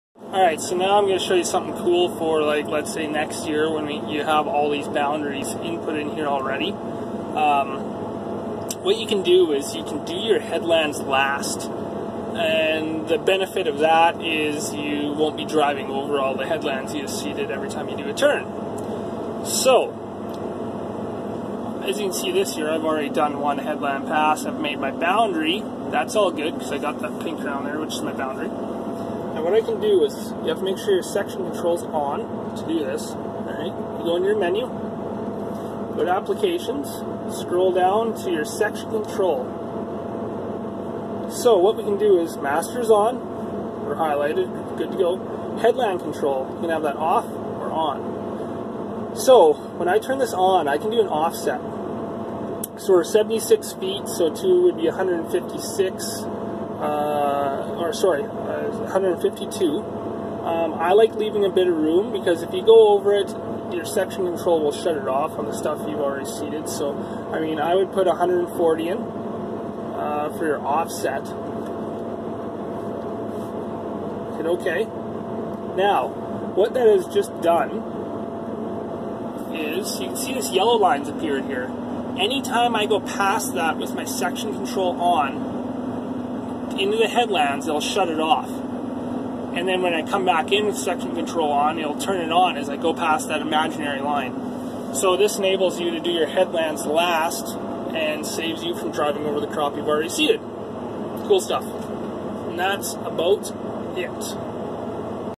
Virtual Headland - John Deere 4600
I make a virtual headland for shutting off sectional control while seeding. This enables me to do the headlands last in a more efficient way.